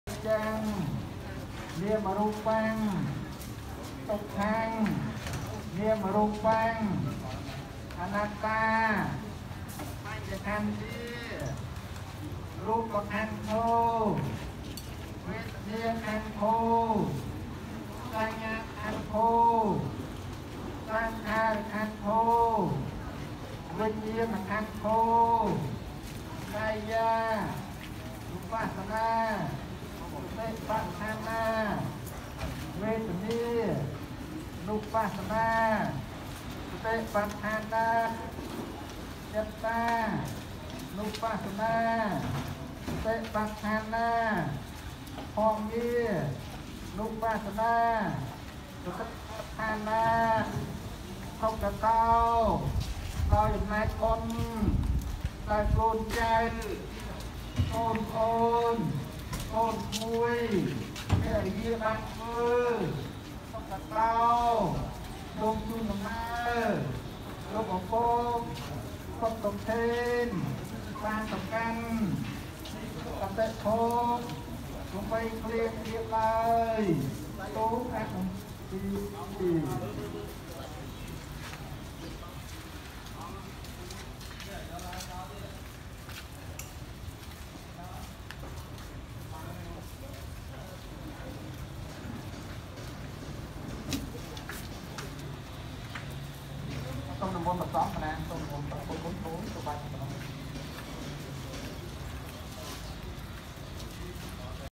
ពិធីបូជាសពឧបាសក់​ សុទ្ធ​ សុខេន​ ២៤.០៩.២០២១
មនុស្សដែកកេីមក្នុងវាលវដ្សាយេីងសុទ្ធតែមានសេចក្តីស្លាប់នៅខាងចុងបំផុតគ្រប់ៗប្រាណមិនថាយេីងមិនថាគេសុទ្ធតែស្លាប់ទាំងអស់។